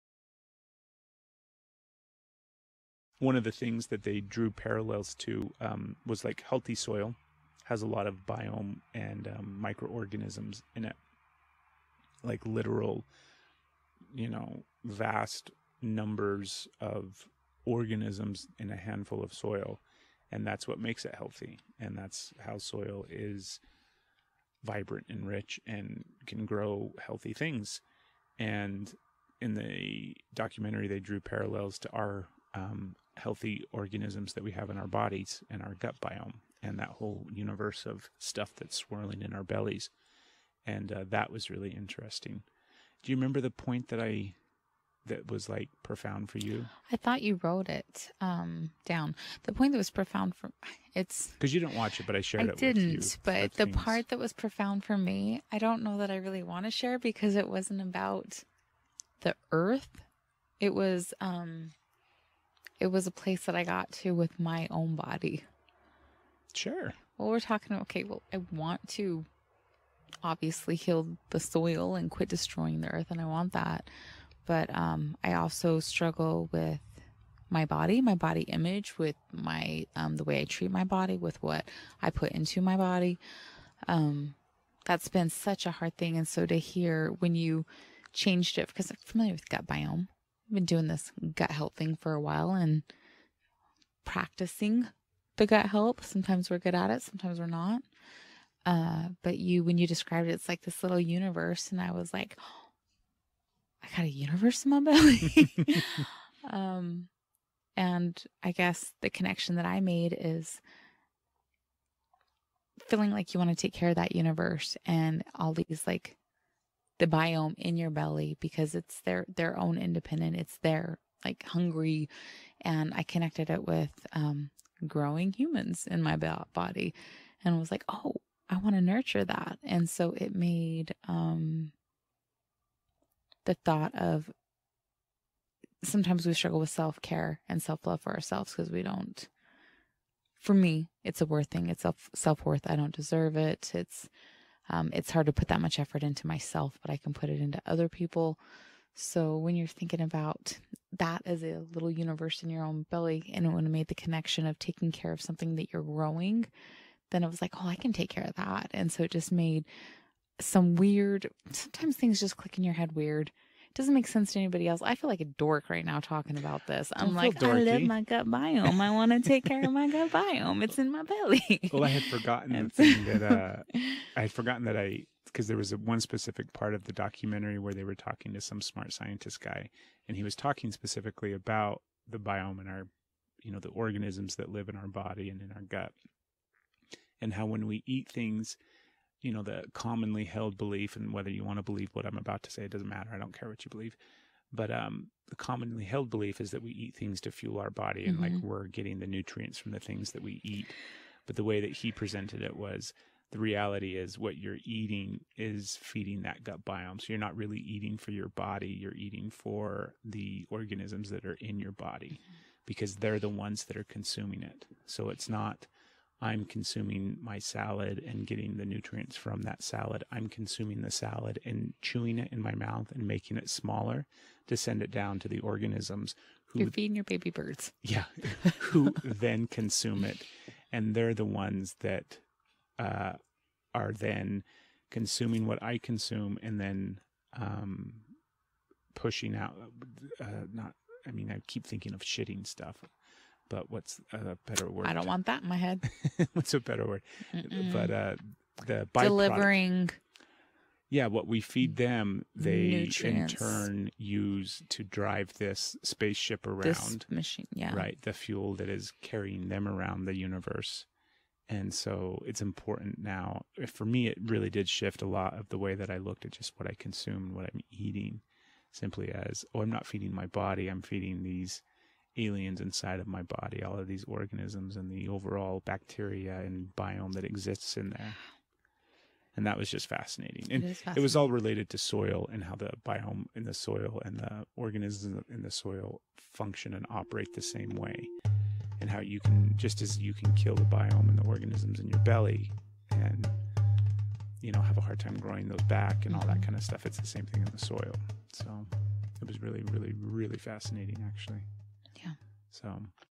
Feeding your gut microbiome.
We discuss the parallels between the human gut microbiome and the soil on our planet. It's a discussion that was sparked by the recent documentary, Kiss the Ground, available on Netflix now.

Go feed your baby birds. Enjoy!

Enter ENERGYISLOVE at checkout and save 20%